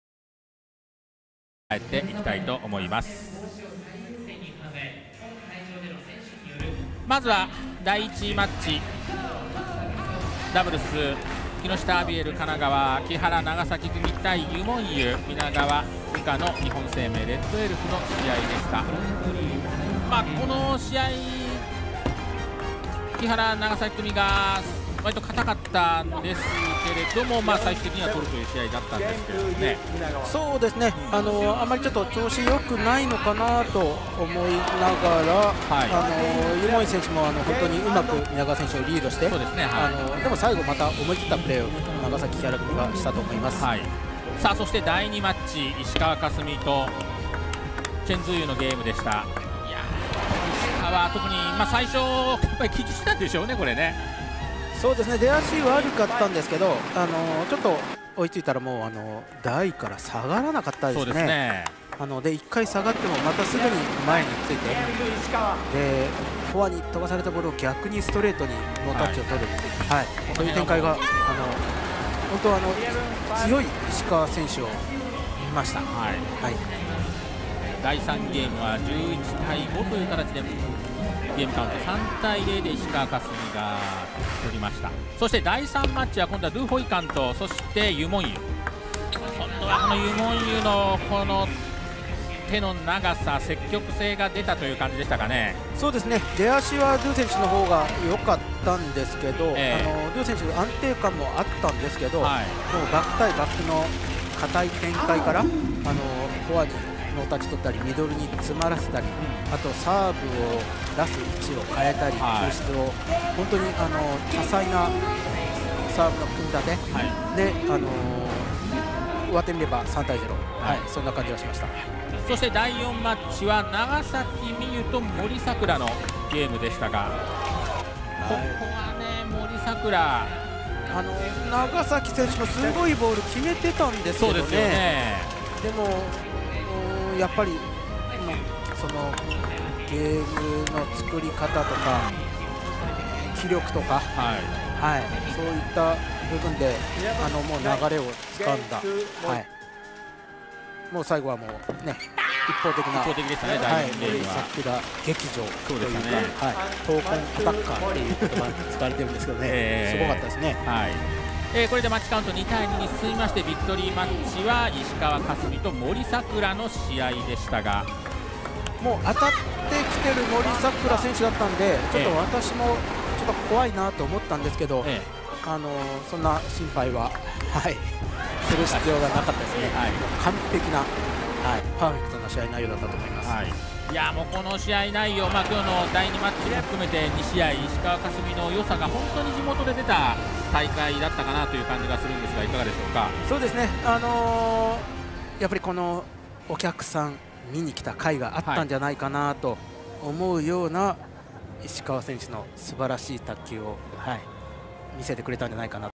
【卓球 Tリーグ公式】 2020年2月11日(火) ノジマＴリーグ 2019-2020シーズン女子 木下アビエル神奈川 VS 日本生命レッドエルフ やまぐちリフレッシュパーク総合体育館
2020年2月11日（火） やまぐちリフレッシュパーク総合体育館

木下アビエル神奈川 3-2 日本生命レッドエルフ
長﨑 美柚・木原 美悠2-1ユ モンユ・皆川 優香
石川 佳純3-0 チェン ズーユ
ドゥ ホイカン0-3ユ モンユ
長﨑 美柚1-3 森 さくら
石川 佳純1-0 森 さくら

審判：安永周二　山根猛寿　山本絹子